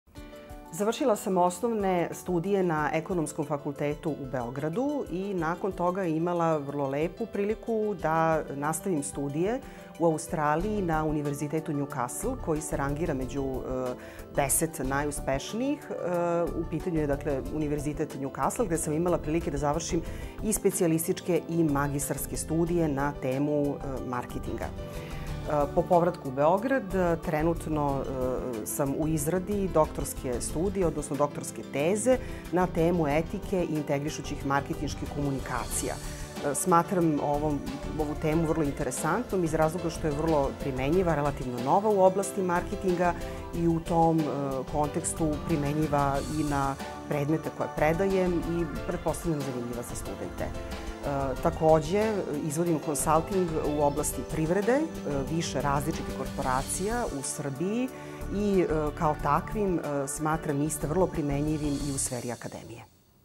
MSc Nevenka Popović-Šević - predavač na BusinessAcademy
Biznis Akademija vam predstavlja Nevenku Popović-Šević, predavača na katedri Marketing.